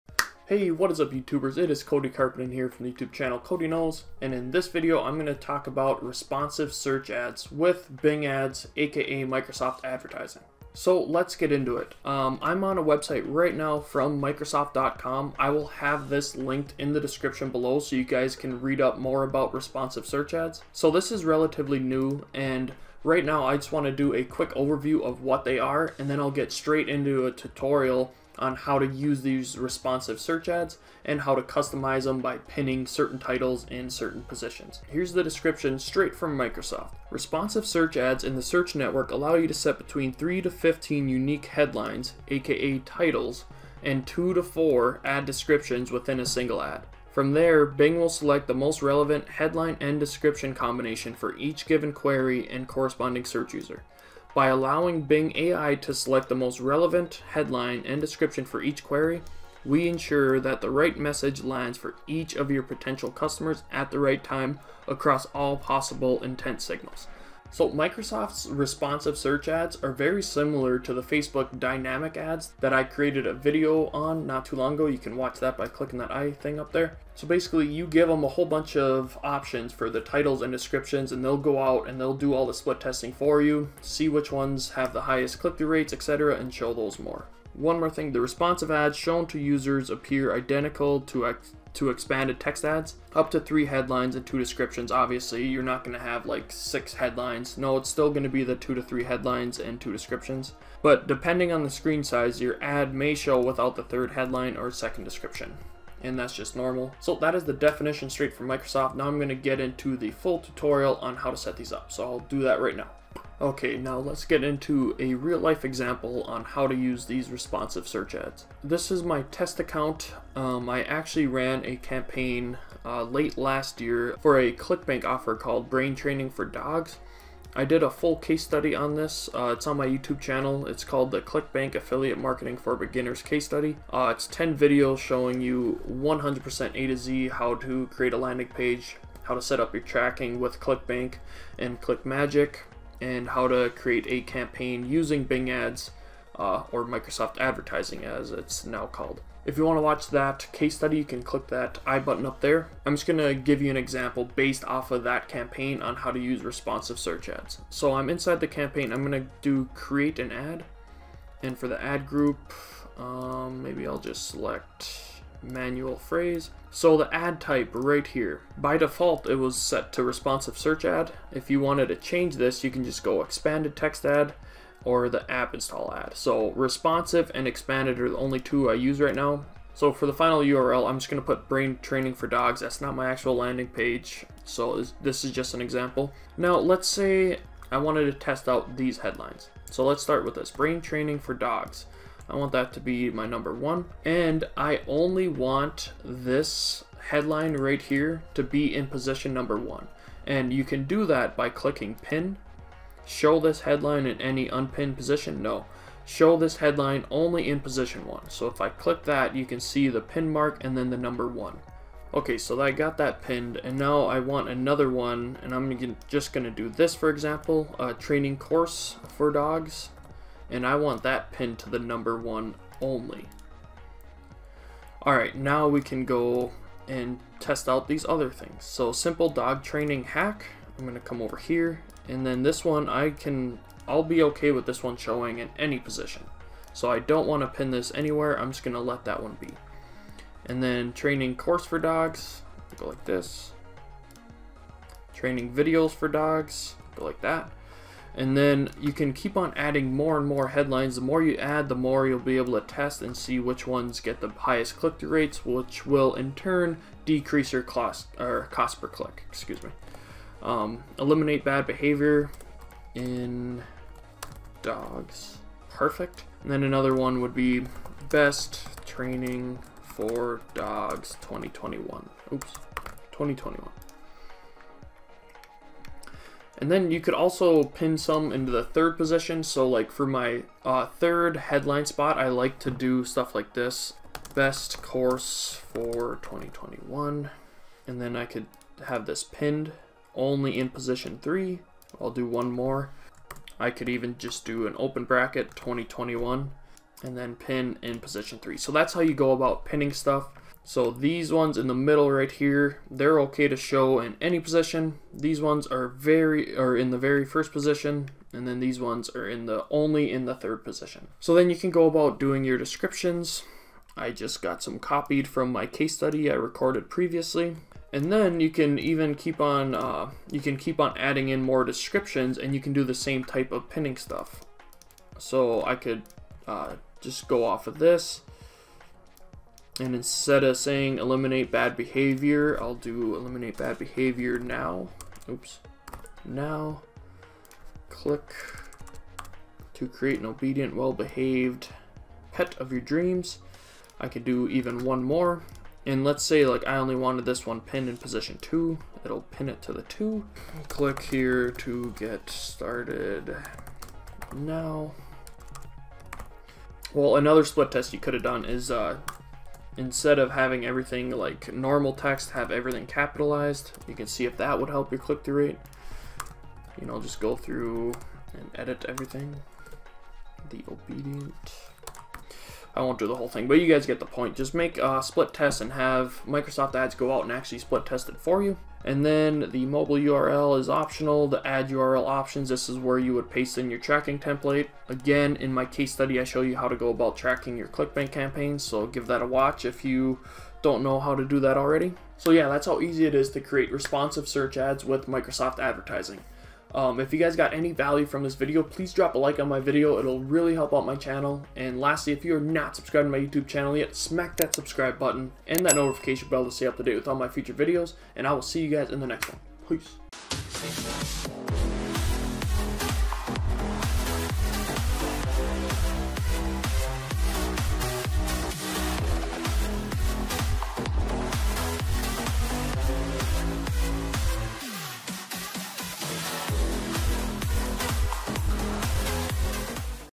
How to Use Responsive Search Ads w/ Bing Ads (Microsoft Advertising)
In this video I will show you a Bing Ads (Microsoft Advertising) tutorial on how to create Responsive Search Ads.

What are Responsive Search Ads?

Basically Bing Ads (Microsoft Advertising) will select the most relevant headlines / descriptions for each given query and corresponding search user. By allowing Bing Ads to select the most relevant headline & description for each query, they ensure that the right message lands for each of your potential customers, at the right time, across all possible intent signals.

This will help increase your CTR and lower your average CPC..